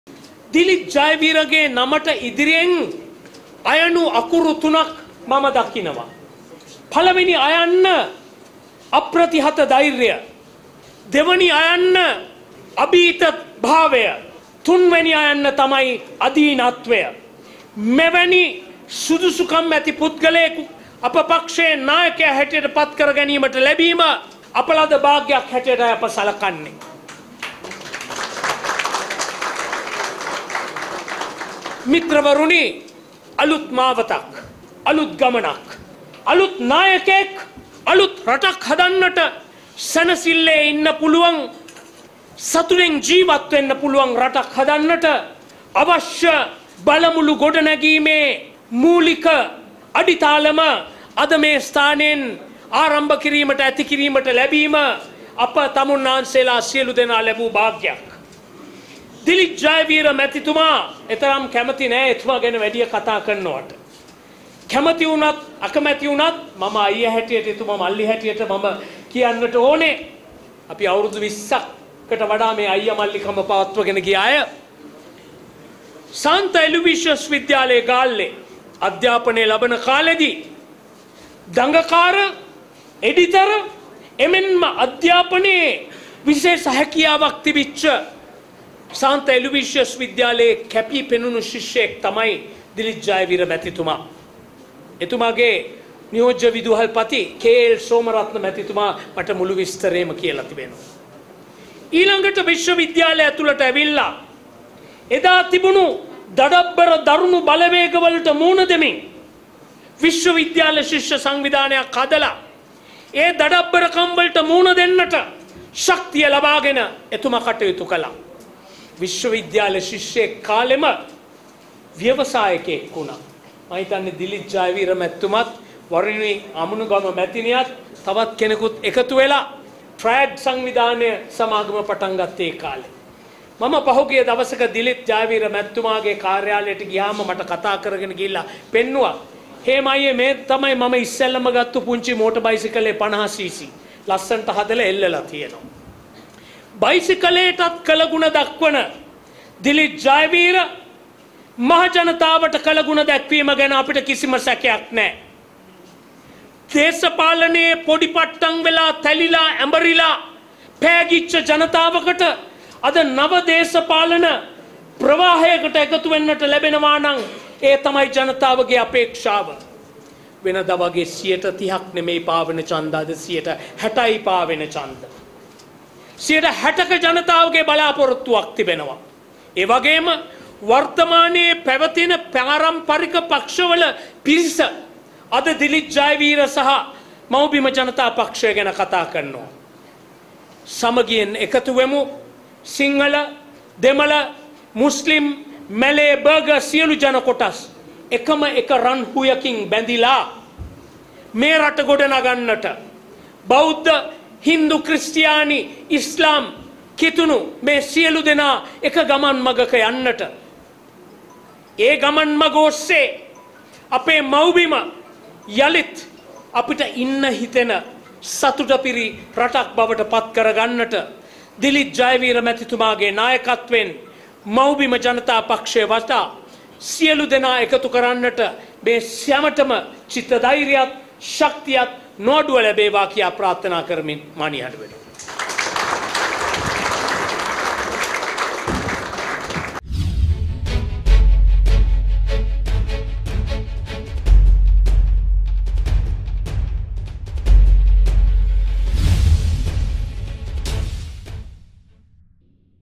දිලිත්ට හේමාගෙන් සහතිකයක් - සැනසිල්ලේ ඉන්න පුලුවන් රටක් හදන්න පුලුවන් නායකයෙක්
දිලිත්ට හේමාගෙන් සහතිකයක් - සැනසිල්ලේ ඉන්න පුලුවන් රටක් හදන්න පුලුවන් නායකයෙක් | පක්ෂ නායකයා වුණේ අපේ භාග්‍යට